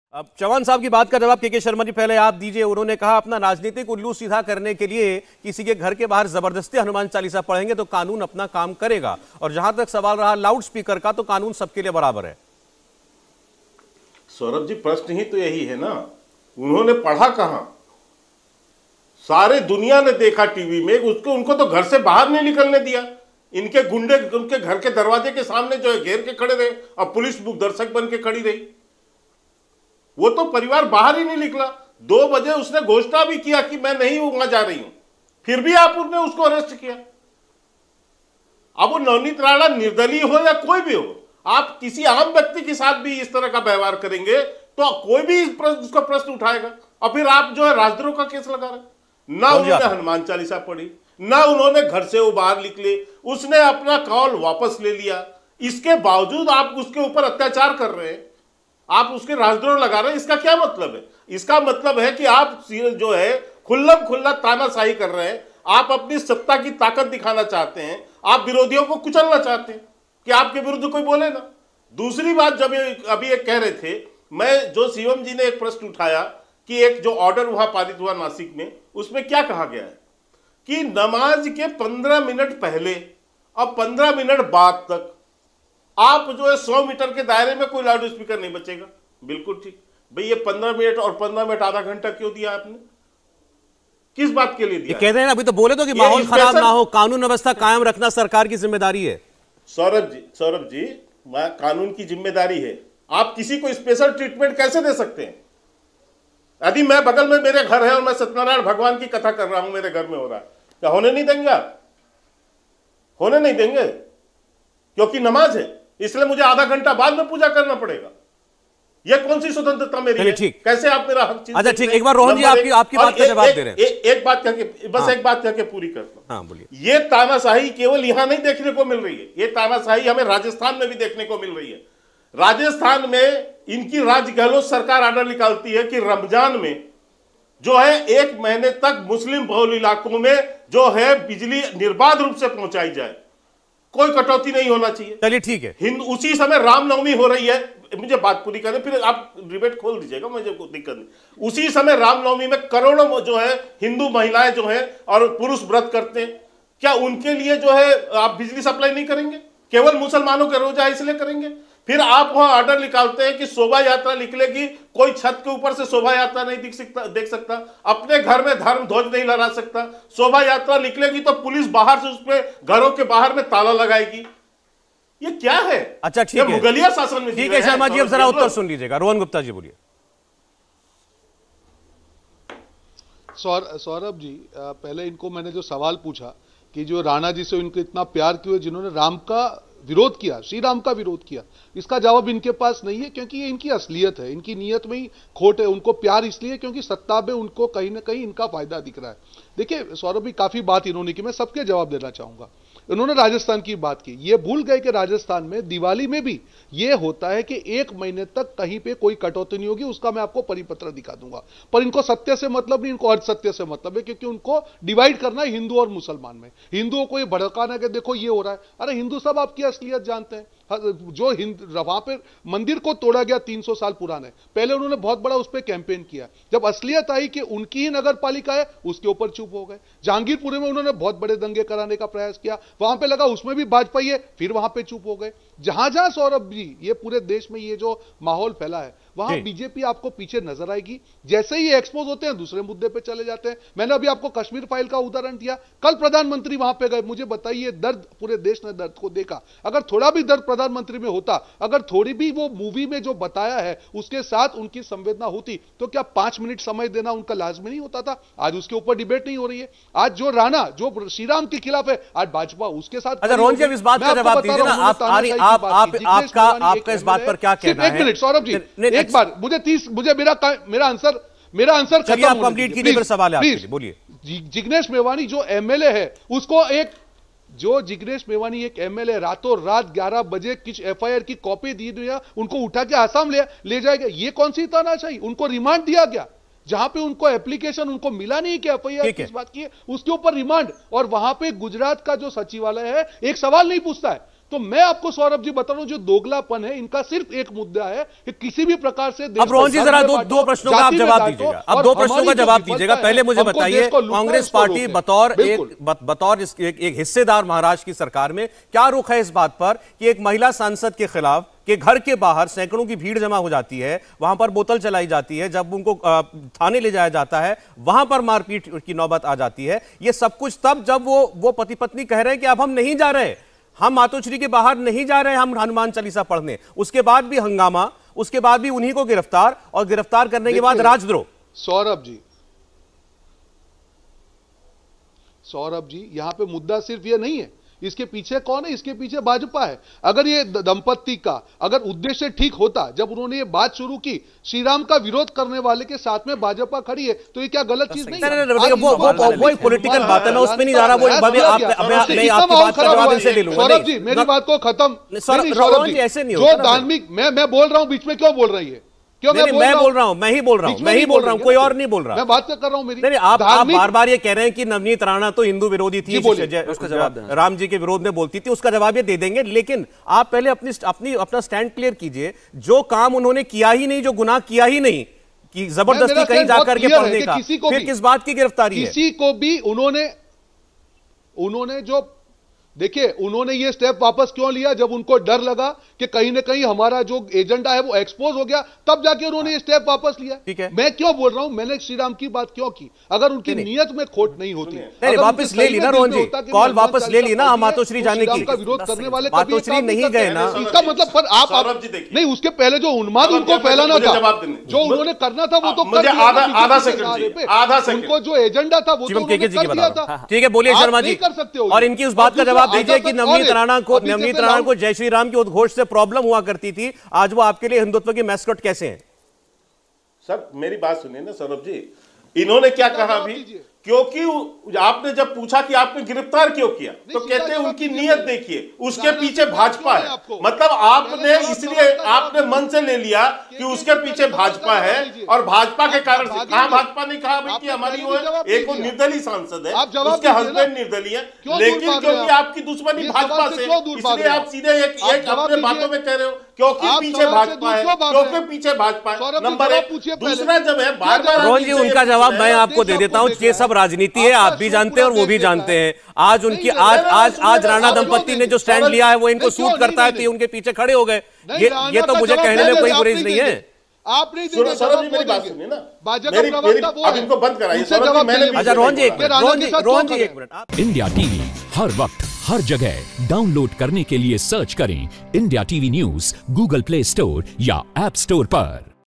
मेरे घर में पूजा नहीं होने देंगे क्योंकि नमाज़ का वक्त है? : KK Sharma, प्रवक्ता BJP ने पूछा
मेरे घर में पूजा नहीं होने देंगे क्योंकि नमाज़ का वक्त है? : KK Sharma, प्रवक्ता BJP ने मुकाबला में कांग्रेस प्रवक्ता से पूछा